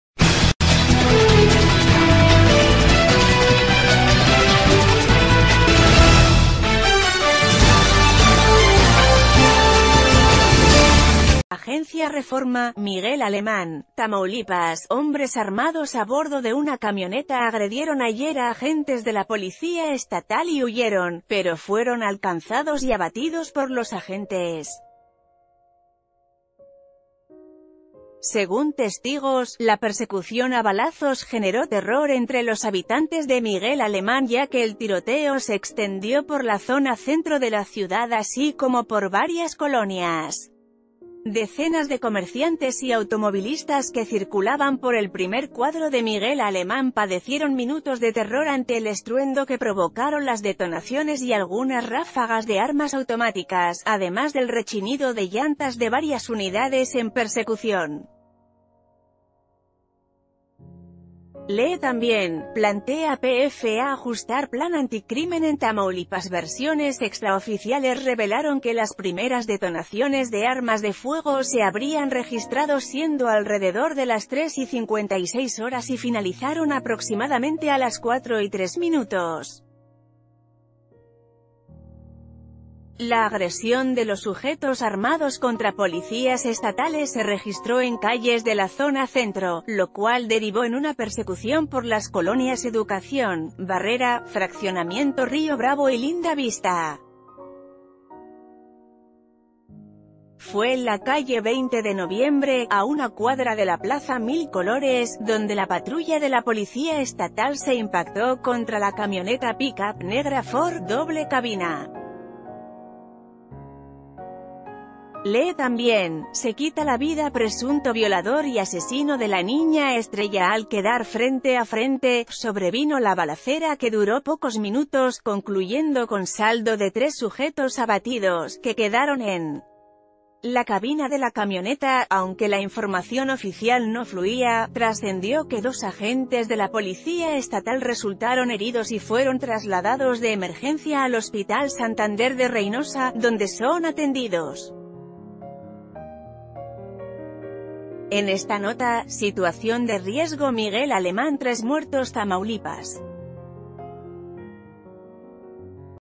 Abaten policías a tres pistoleros en Tamaulipas
Agencia Reforma. MIGUEL ALEMÁN, Tamaulipas.-Hombres armados abordo de una camioneta agredieron ayer a agentes de la Policía Estatal y huyeron, pero fueron alcanzados y abatidos por los agentes. Según testigos, la persecución a balazos generó terror entre los habitantes de Miguel Alemán ya que el tir...